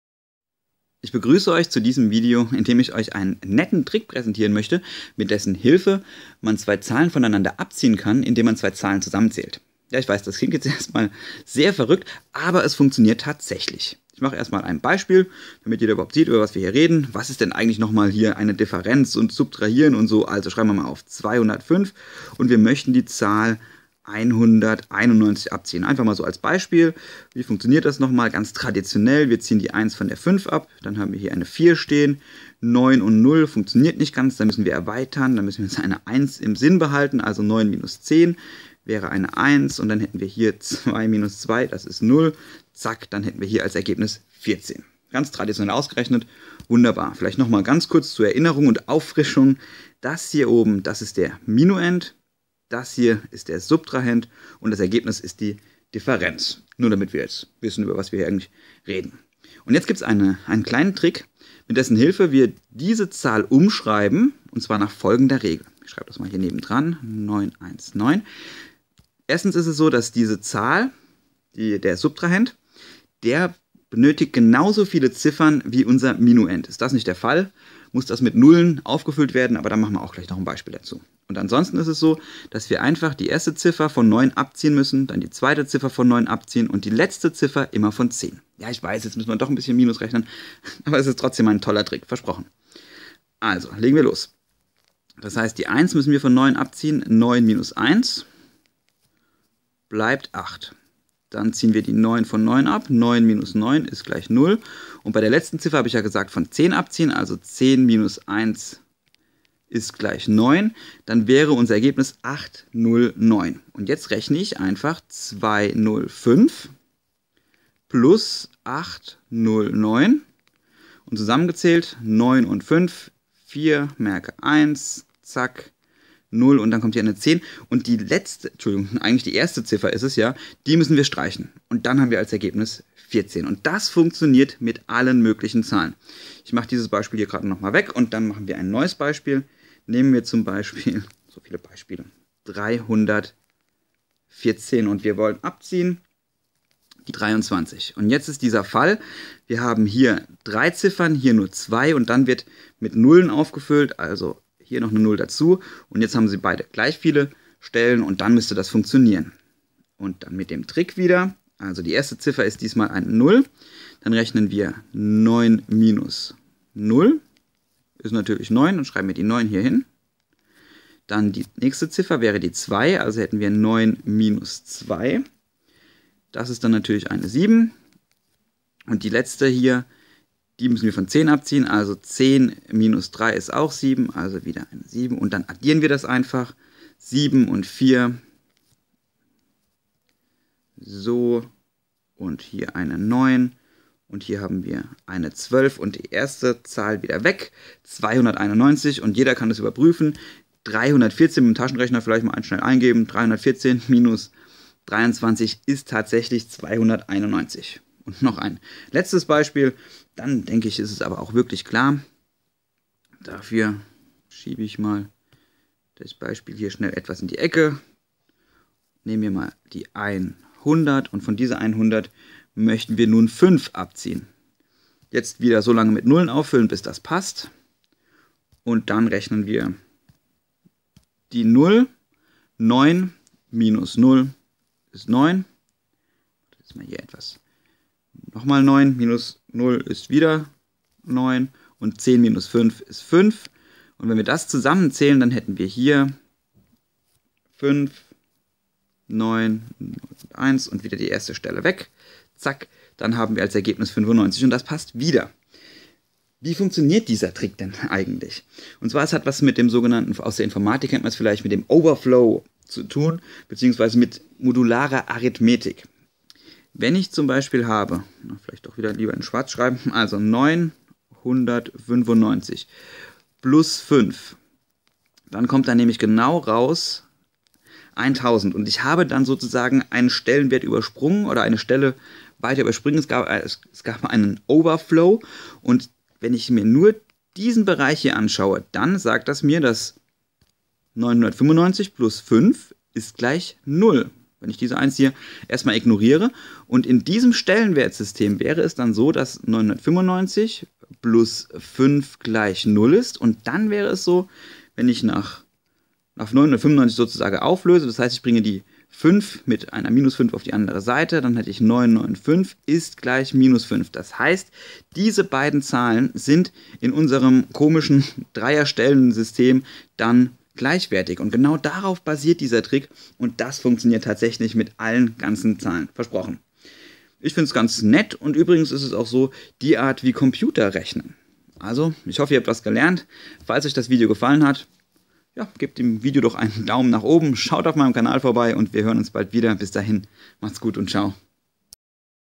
Mathe Trick: Addition durch Subtraktion
Ein netter Trick mit dessen Hilfe man zwei Zahlen voneinander abziehen kann indem man zwei Zahlen zusammenzählt.